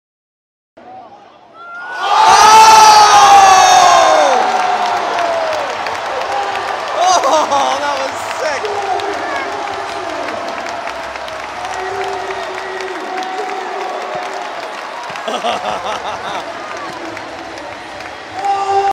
Ridiculous Jordan Clarkson Dunk at Mizzou Madness
Mike Dixon tossing the ball from the stands...